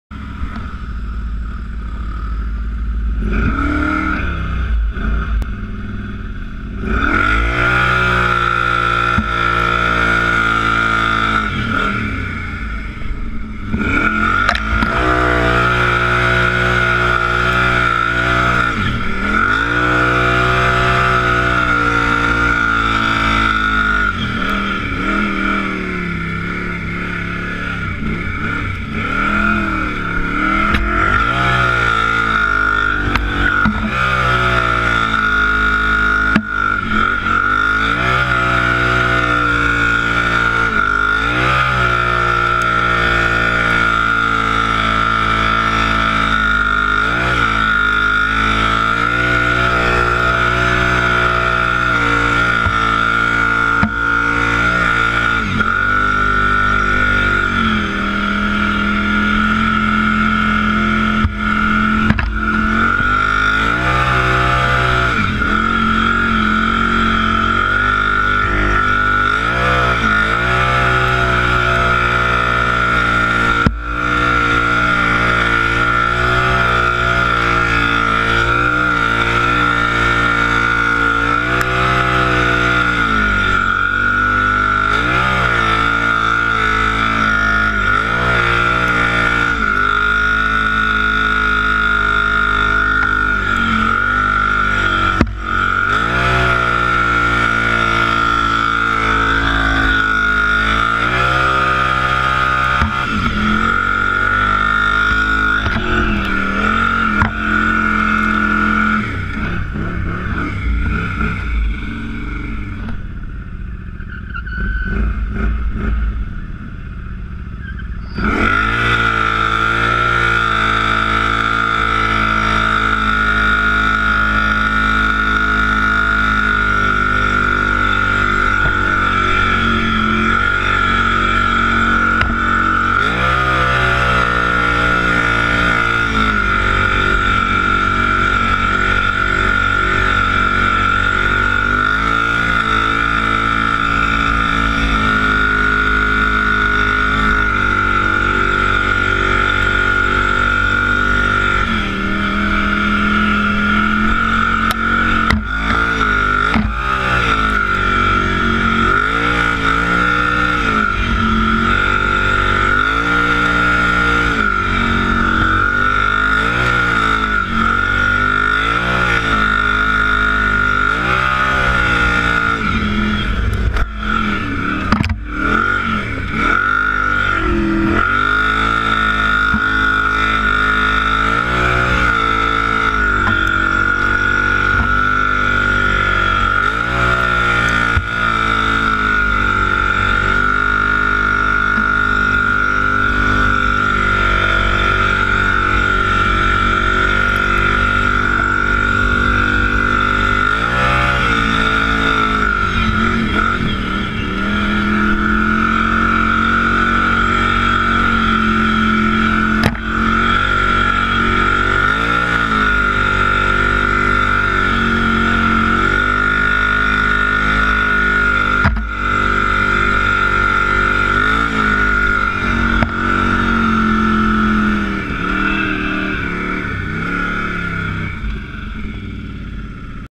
840 Brute - Sands Road Run - 4
Headed to Ryans farm. March 3, 2013

Video taken with a Go Pro hero 3 Black Edition, 1080p 30 fps.